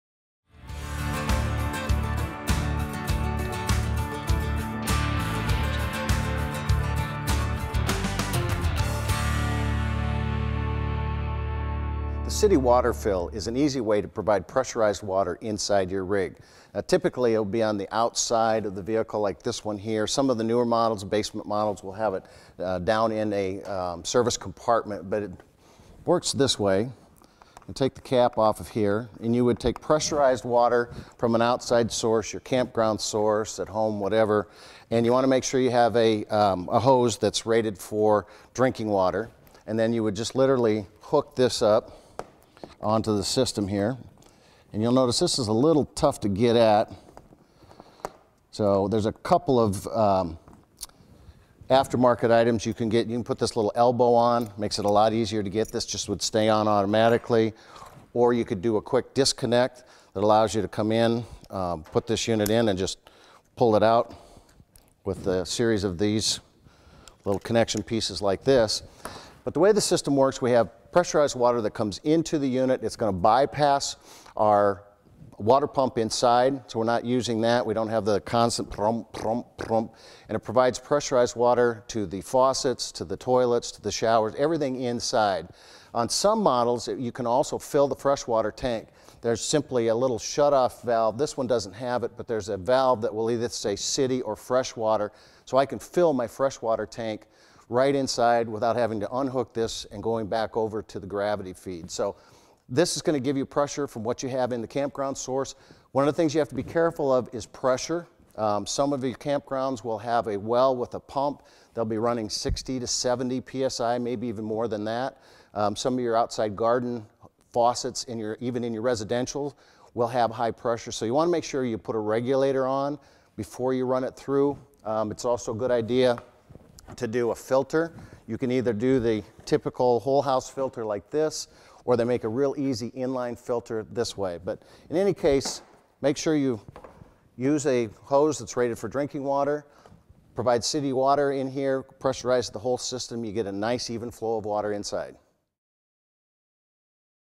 How To Properly Connect The RV City Water Inlet
Looking for the latest RV tips, techniques, and travel inspiration? Sign up for the free RVLR newsletter today!

The RV city water inlet is an easy way to provide pressurized water into your RV unit.